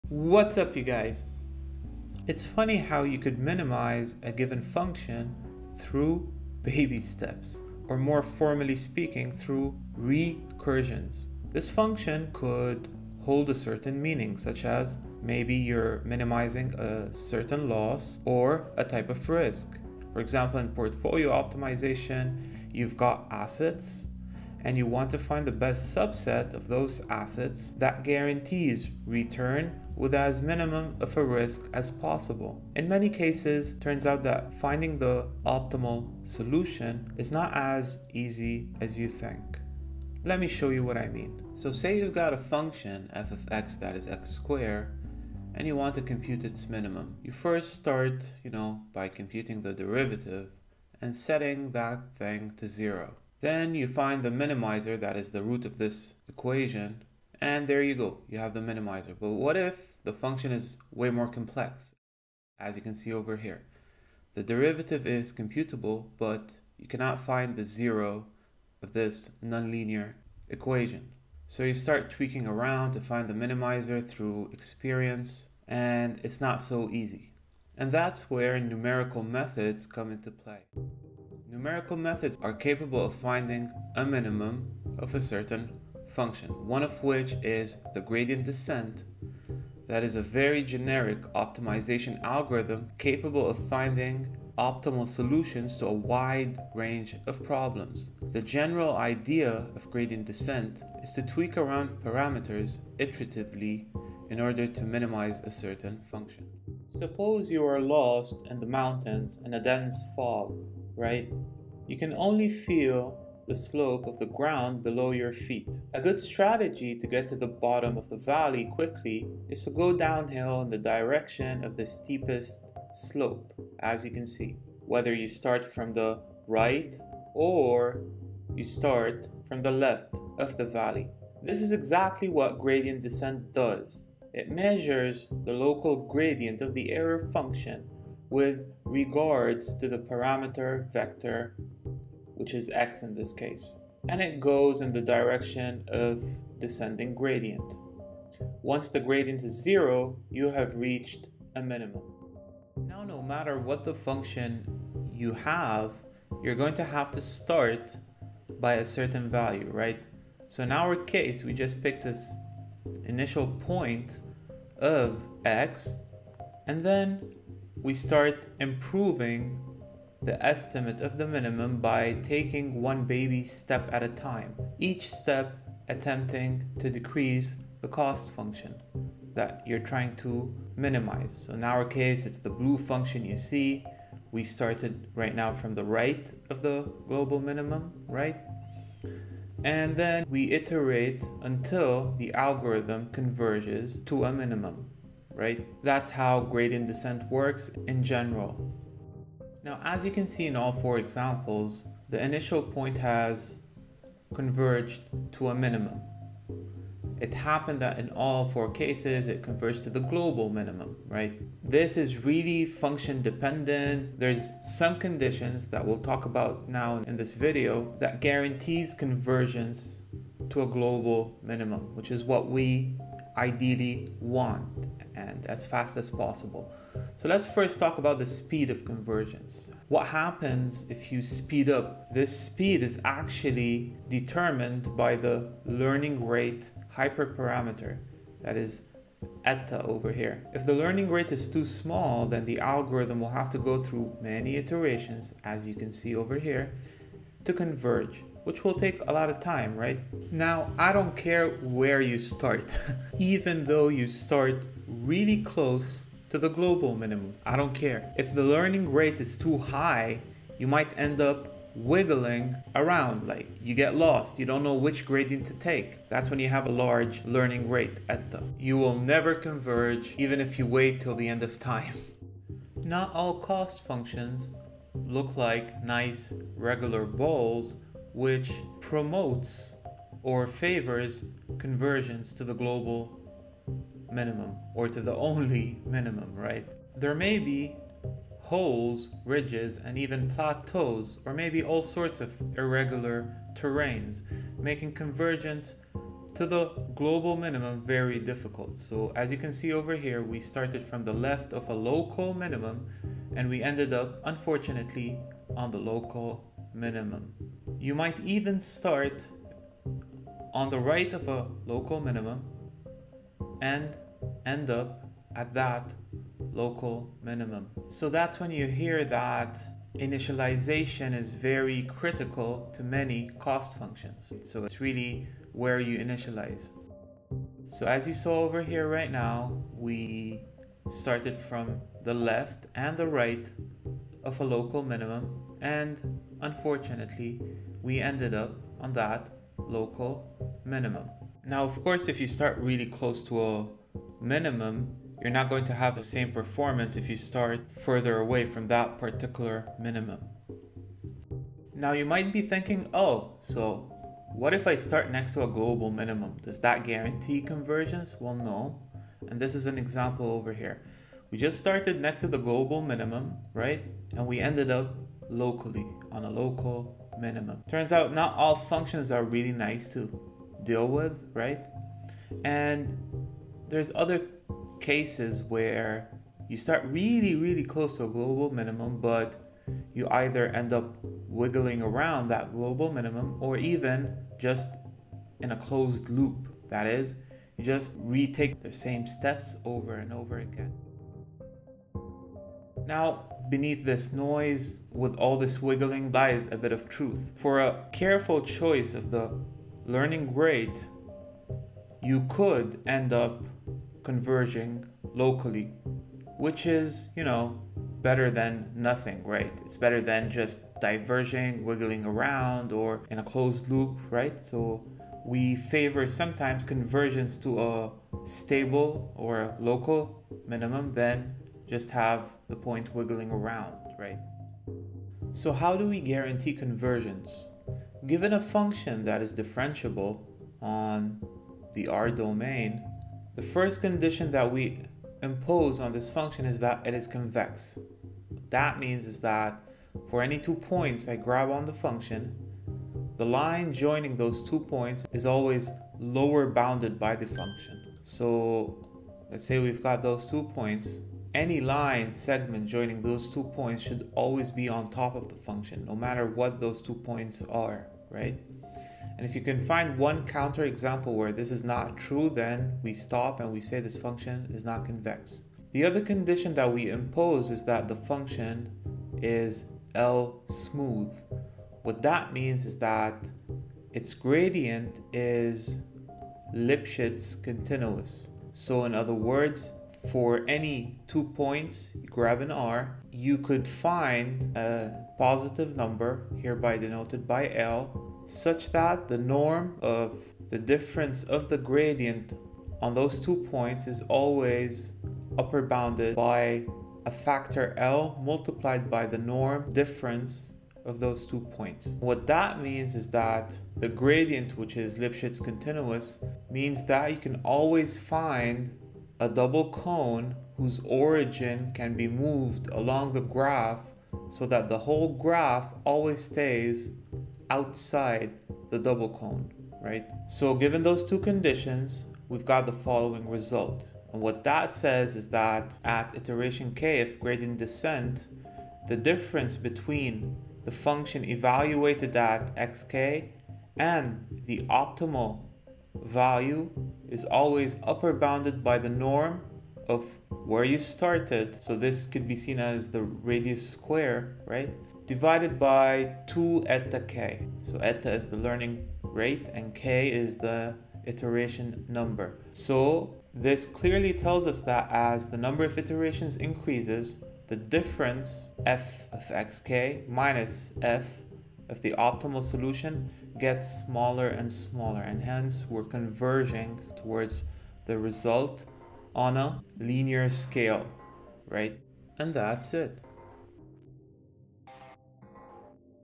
Gradient Descent under 10 minutes feat Broid
All you need to know about one-dimensional Gradient Descent under 10 minutes. Enjoy.

👁‍🗨 Speak up and comment, I am all ears.

🎵 Thanks freesound.org and Erokia and all the users in this list for the following sounds I have used;